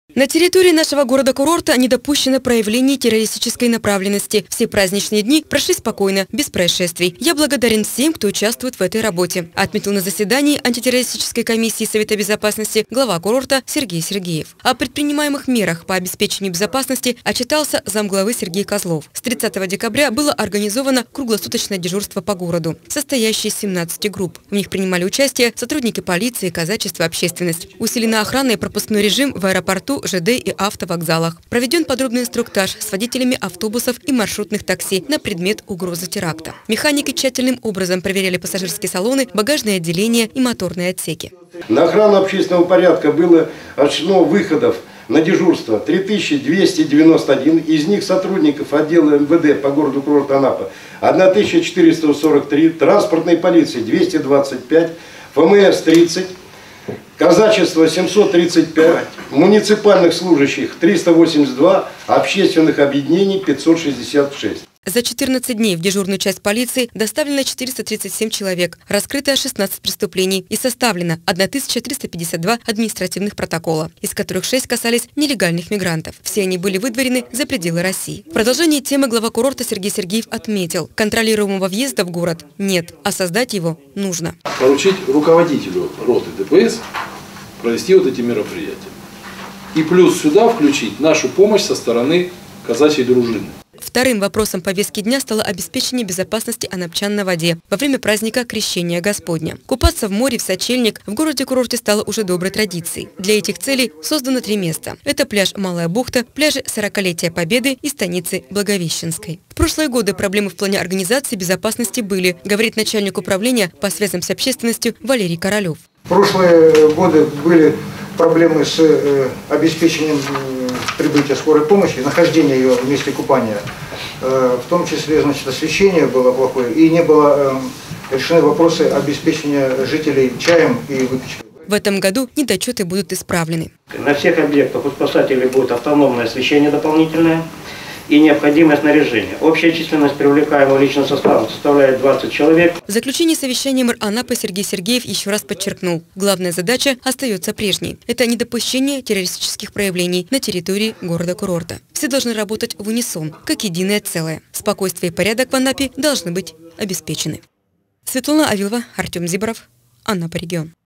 В Анапе все праздничные дни прошли спокойно, без происшествий
Совет безопасности